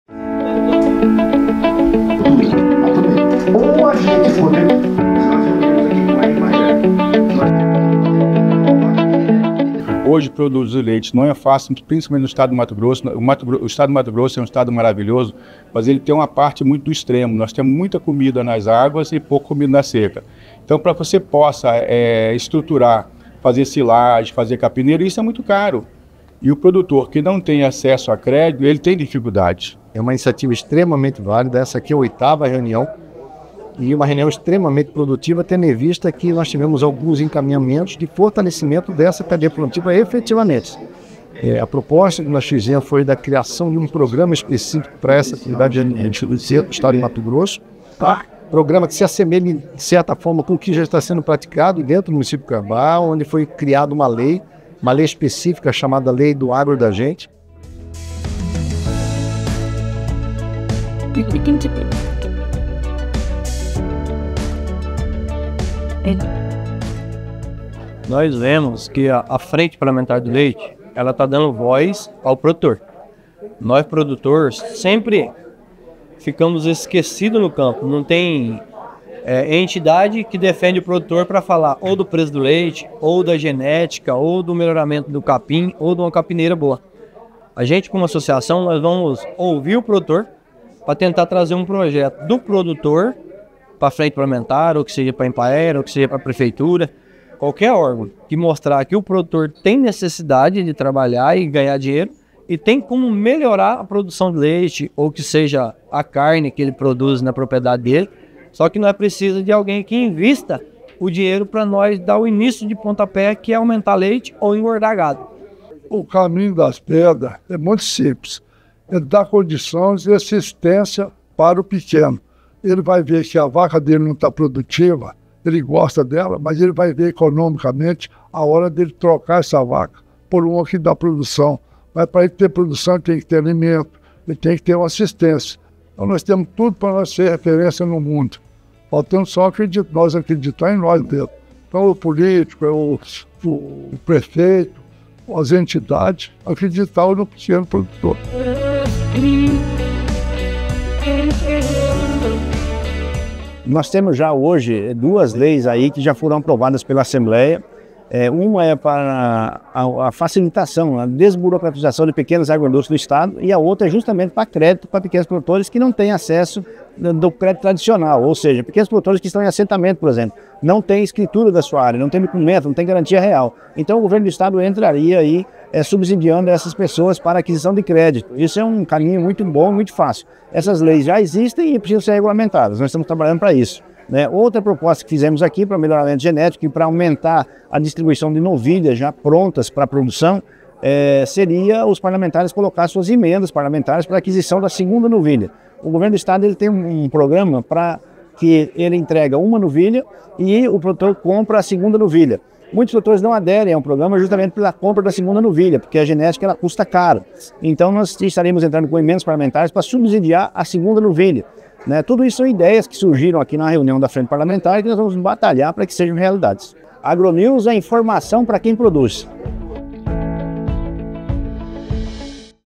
Frente Parlamentar do Leite propõe Grupo de Trabalho para impulsionar produção em Mato Grosso
Mato Grosso pretende se tornar a maior bacia leiteira do país. Melhoramento genético, facilidade de acesso a créditos e assistência técnica qualificada, serão implementadas como estratégias para alavancar o setor leiteiro no estado